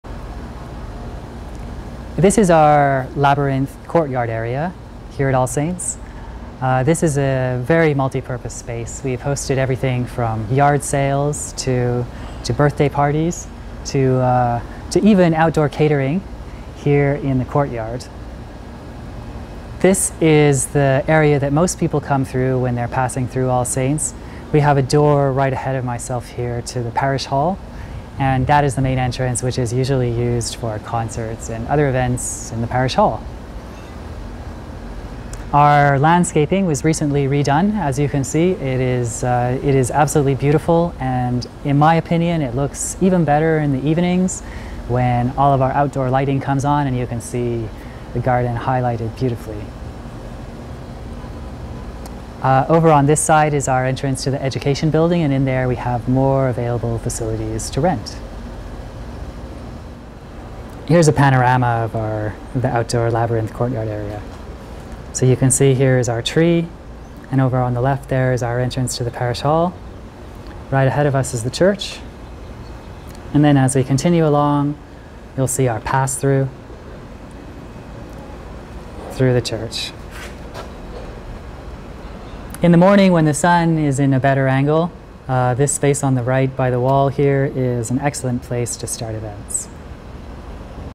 The Courtyard at All Saints Palo Alto
The Courtyard at All Saints Palo Alto is available to rent for your next outdoor event.